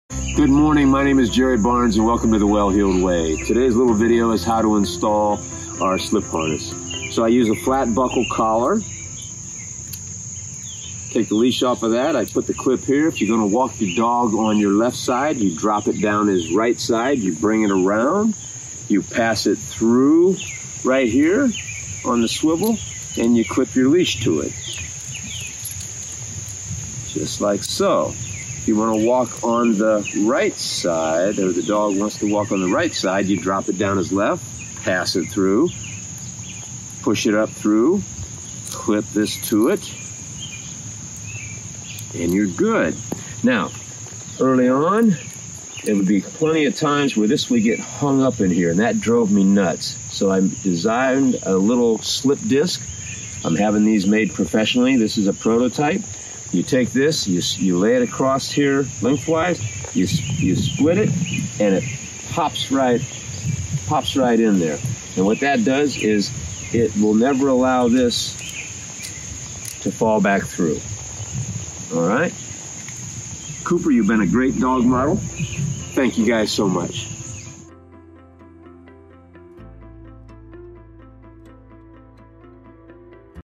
How to Install The Well Heeled Way® Custom Slip Harness
The Well Heeled Way® Custom-Fit Slip Harness was inspired and designed after decades of dog training. I had no idea how incredibly well this would work on ALL dogs, but we soon found out how game-changing this training hardware could become.

Online Dog Training Course at thewellheeledway.com